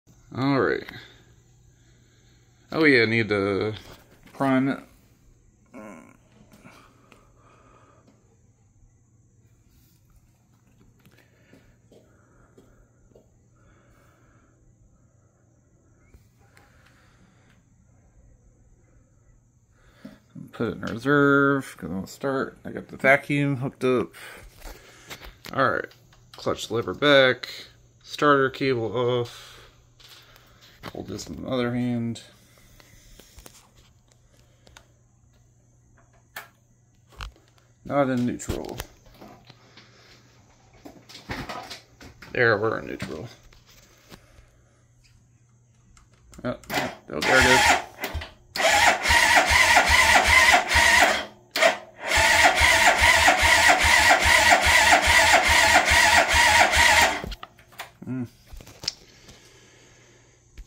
First attempt to start 2005 gz250 after rebuild and engine back on bike
Replaced the ebay piston with oem one new rings and original cylinder and nasaki head.
Replaced starter clutch internals
New battery and this was fully charged
Just filled with oil and fresh nonethanol gas.